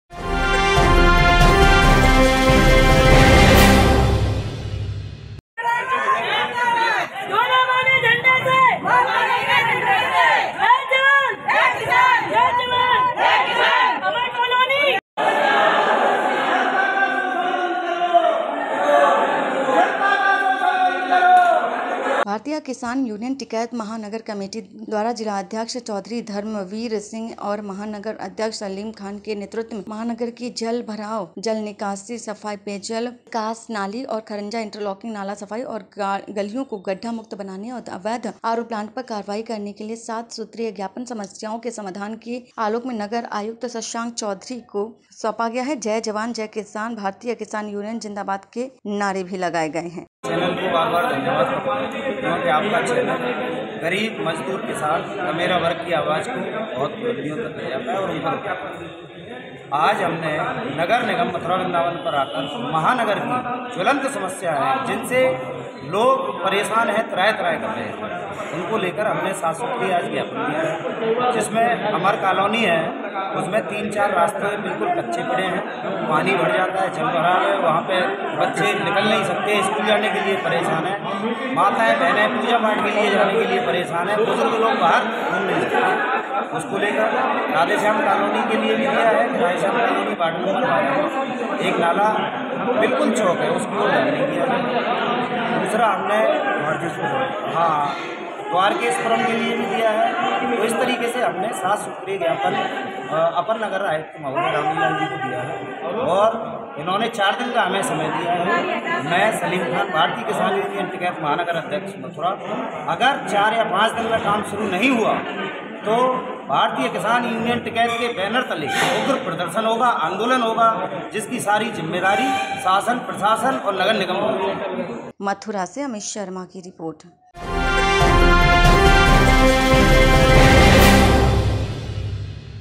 Mathura : भारतीय किसान यूनियन चौधरी धर्मवीर सिंह ने प्रदर्शन कर ज्ञापन सौपा | JANKANPUR24X7
Your Queries:-भारतीय किसान यूनियन टिकैत महानगर कमेटी द्वारा जिला अध्यक्ष चौधरी धर्मवीर सिंह एवं महानगर अध्यक्ष सलीम खान के नेतृत्व में महानगर की जल भराव जल निकासी सफाई पेयजल प्रकाश नाली खरंजा इंटरलॉकिंग नाला सफाई एवं गलियों को गड्ढा मुक्त बनाने ,अवैध आरो प्लांट पर कार्रवाई हेतु सात सूत्रीय ज्ञापन समस्याओं के समाधान के आलोक में नगर आयुक्त शशांक चौधरी जी को जय जवान जय किसान भारतीय किसान यूनियन जिंदाबाद के गगन भेदी नारे लगाकर प्रदर्शन कर ज्ञापन  सोपl
प्रदर्शन के दौरान जिला अध्यक्ष धर्मवीर सिंह ने नगर निगम प्रशासन की उदासीनता एवं घनी मिश्रित आबादी में समस्याओं के निराकरण में रुचि नहीं दिखाई जाने पर आक्रोश व्यक्त किया इस मौके पर महानगर अध्यक्ष सलीम खान एवं लोकेश कुमार राय ने संयुक्त रूप से महानगर के कुछ विशेष क्षेत्र राधेश्याम कॉलोनी में मुख्य मार्ग पर नाला सफाई गड्ढा मुक्त सड़क बनाने अमर कॉलोनी में गलियों का निर्माण भागलपुर वार्ड नंबर 16 वार्ड नंबर 59 प्रभु नगर में प्रभु नगर में इंटरलॉकिंग सड़क का निर्माण आदि की मांग करते हुए चेतावनी दी अगर 7 दिन के अंदर कार्य शुरू नहीं हुए तो महानगर कमेटी अनिश्चितकाल के लिए धरना प्रदर्शन करेगी जिसकी जिम्मेदारी नगर निगम प्रशासन की होगी प्रदर्शन कार्यक्रम नगर आयुक्त शशांक चौधरी के आश्वासन के बाद समाप्त हुआ प्रदर्शन के लोकेश कुमार राही व महानगर अध्यक्ष सलीम खान  सदर तहसील अध्यक्ष भगवान दास मिश्रा पश्चिमी उत्तर प्रदेश उपाध्यक्ष राजेंद्र सिंह रावत नगर अध्यक्ष वृंदावन विनोद राजपूत नगर सचिव मथुरा असलम कुरैशी चांद कुरैशी रामबाबू दीपू राजकुमार मामा जिला अध्यक्ष चौधरी धर्मवीर सिंह मंडल सचिव राकेश चौधरी जिला उपाध्यक्ष राजवीर नेताजी ओमप्रकाश प्रधान जी अर्जुन सिंह प्रधान जी रामकिशन कृष्ण कुमार निजाम खान आलमगीर जीतू झा द्रोणाचार्य कुंतल सीमा शर्मा राजवती ज्ञान देवी कलावती सुभद्रा गीत सुमन विवेक सैकड़ो की संख्या में लोग मौजूद रहे